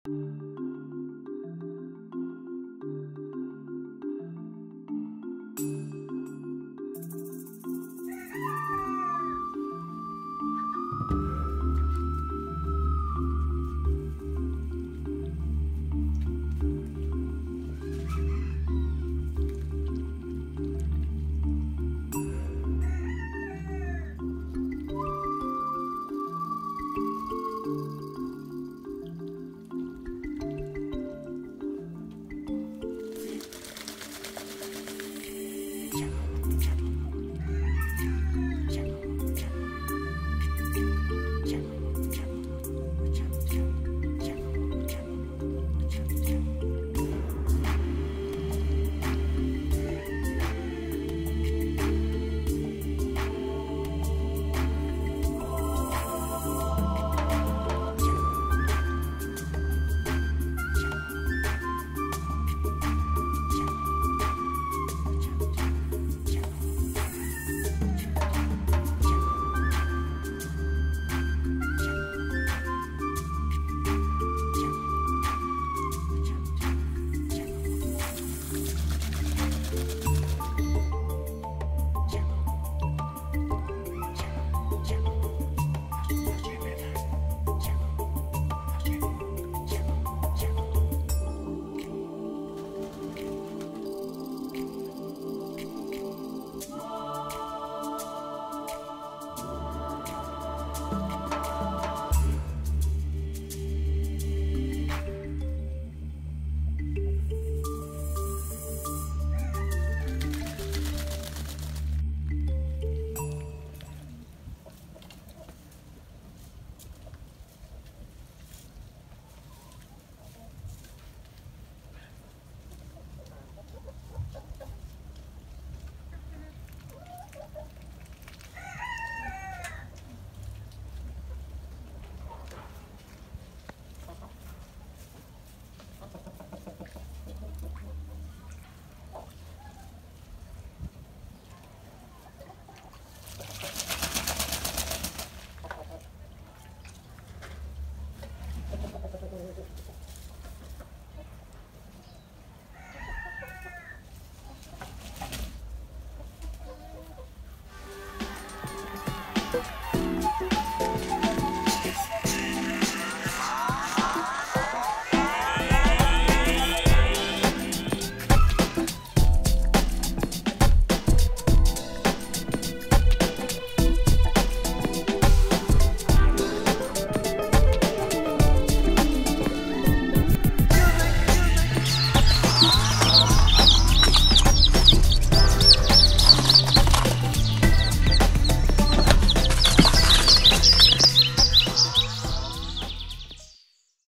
Small Bird's Pond
Do you know how do they clean their feather? The big beautiful white duck is enjoying his small pond.